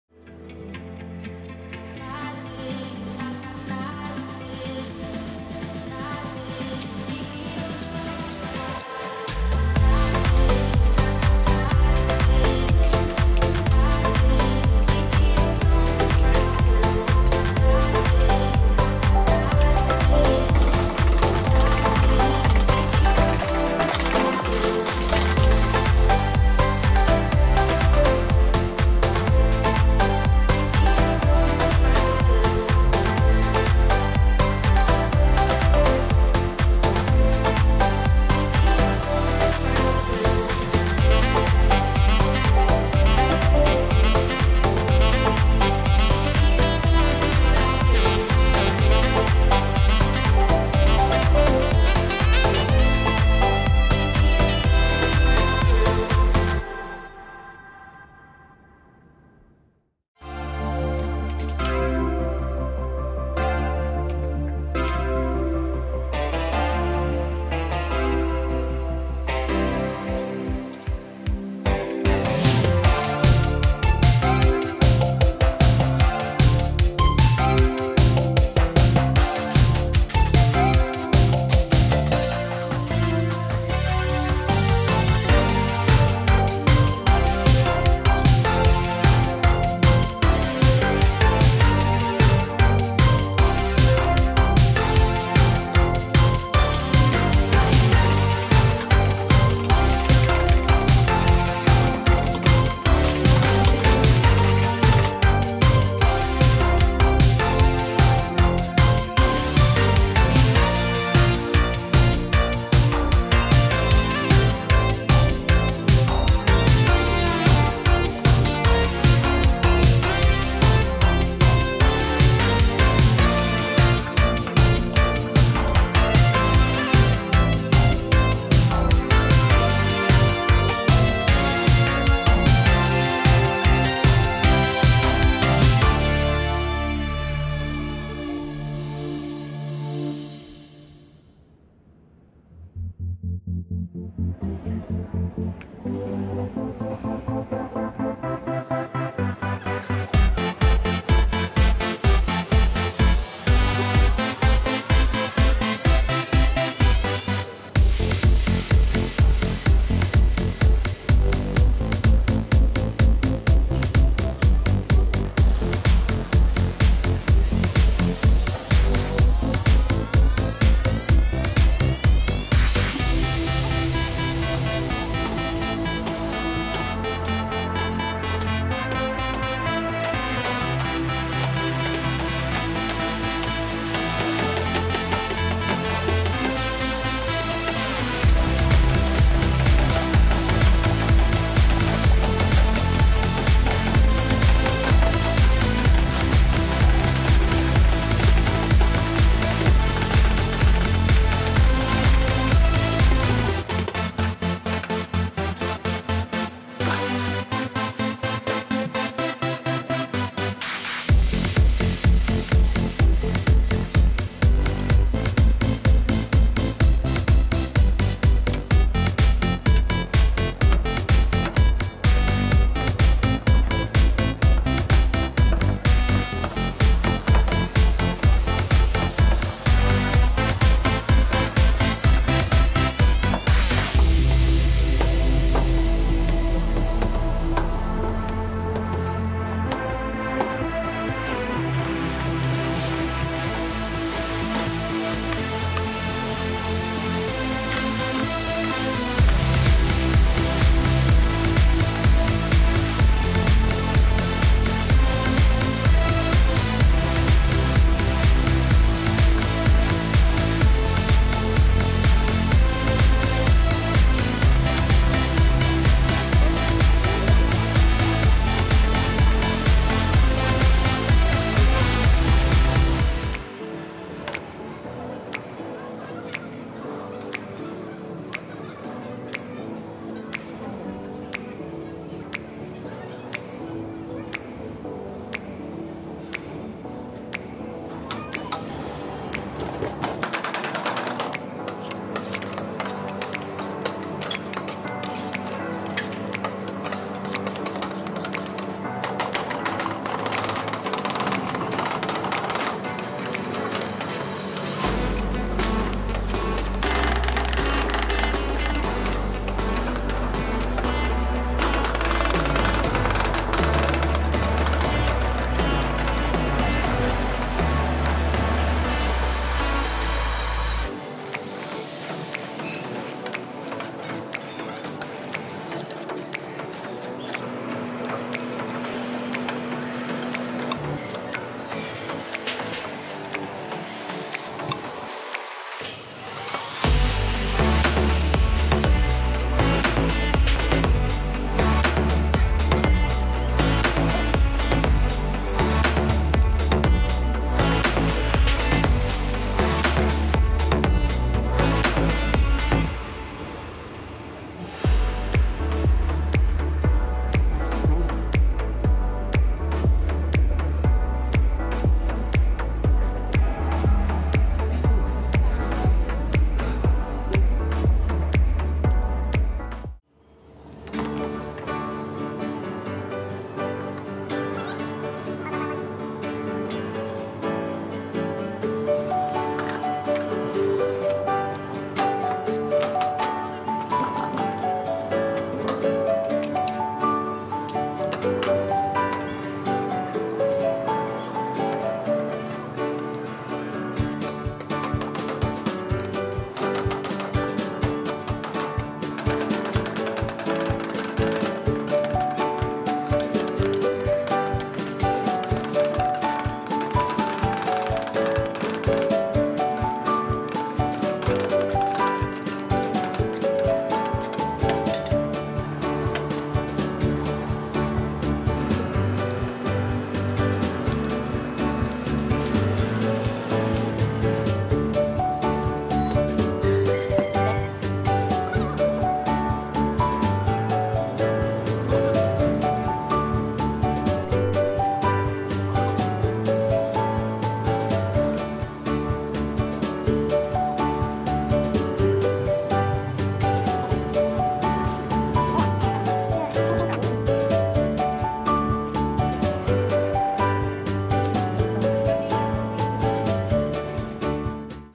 Rainbow Easter Egg Street IceCream | Colorful needle cream
00:00 Colorful needle cream
01:00 Rainbow Easter Egg Street IceCream
02:24 Mini Tomato iceCream
04:33 Blue Macaroon IceCream
06:10 Macaroon IceCream